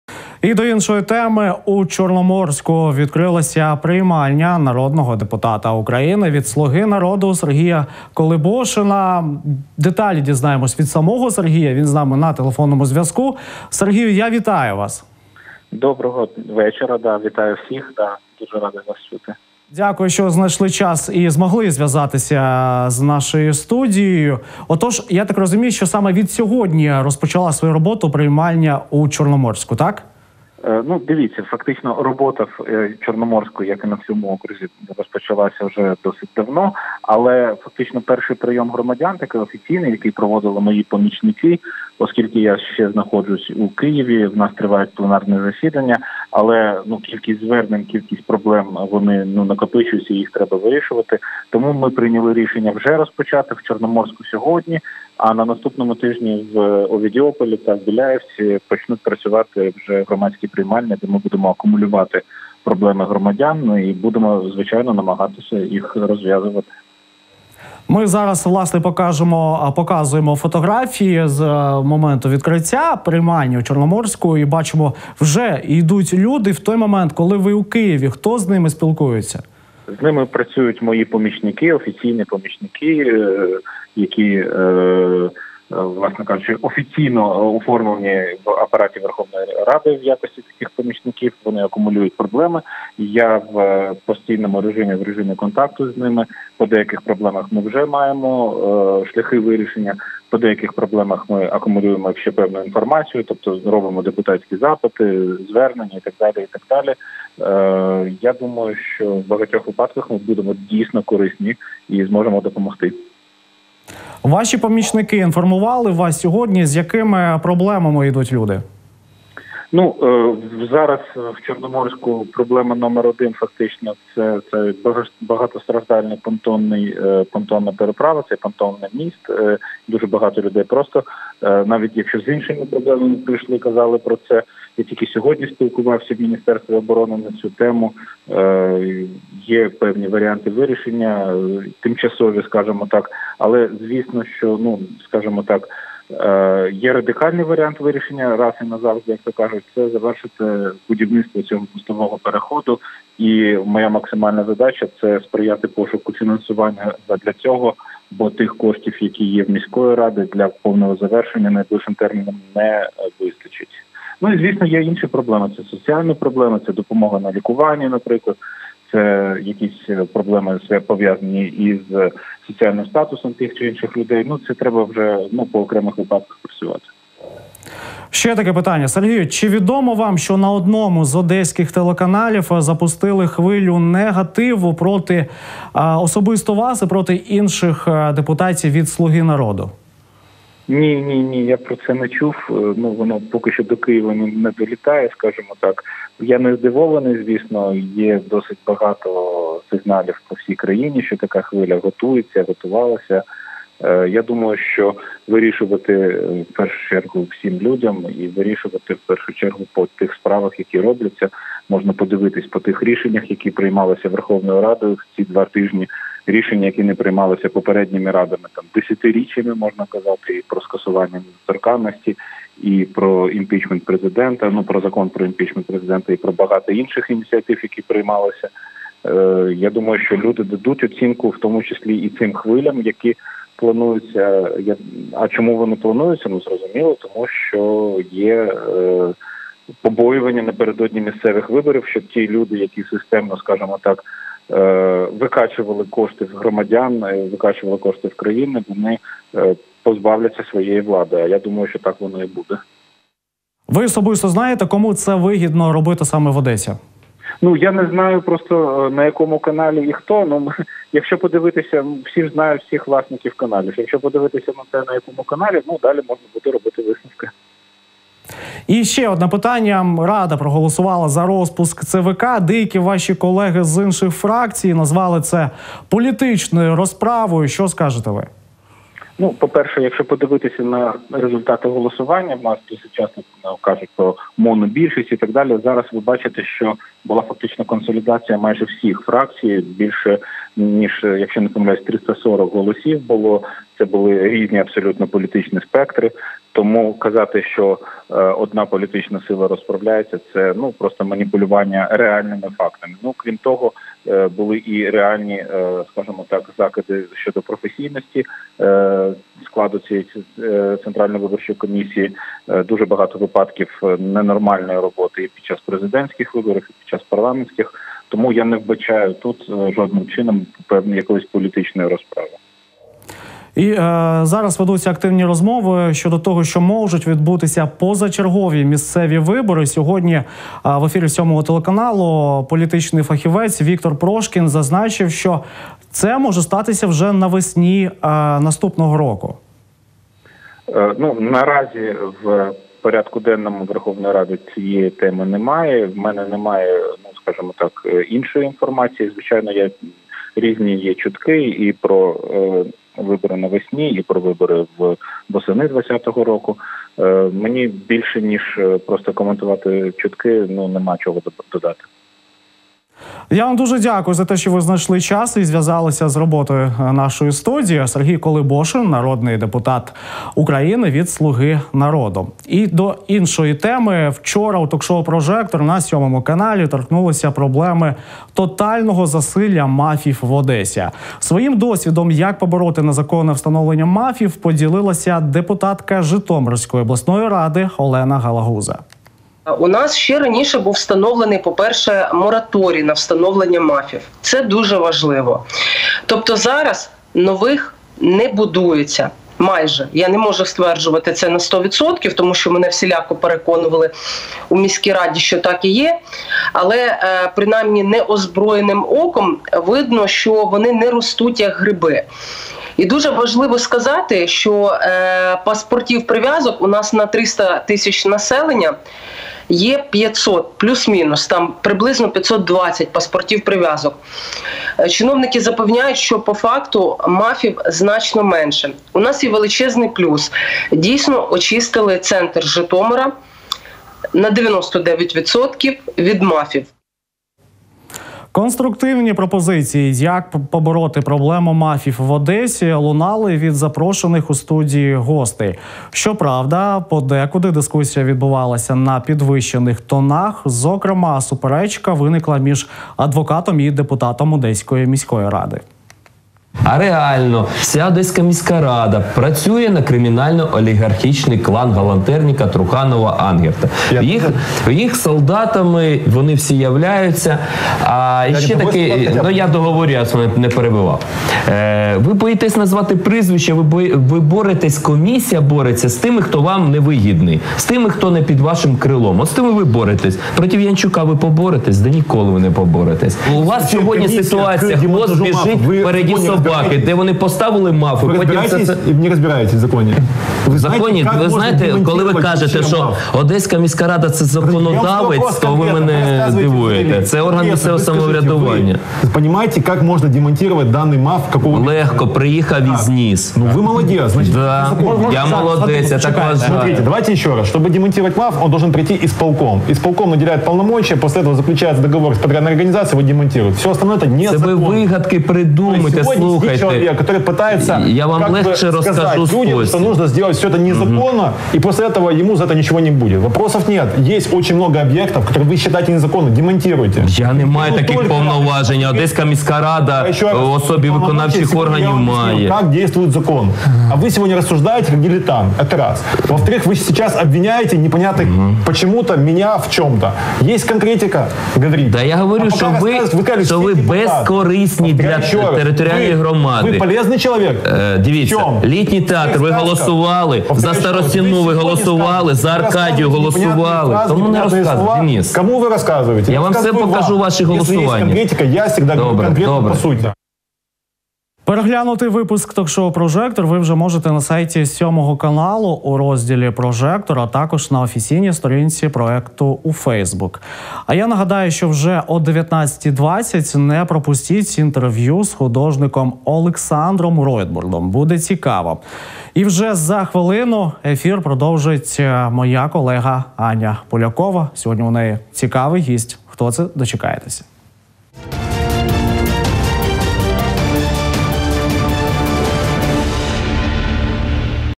У Чорноморську відкрилась приймальня нардепа Сергія Колебошина
телефоном:
Сергій Колебошин, народний депутат України (Слуга народу)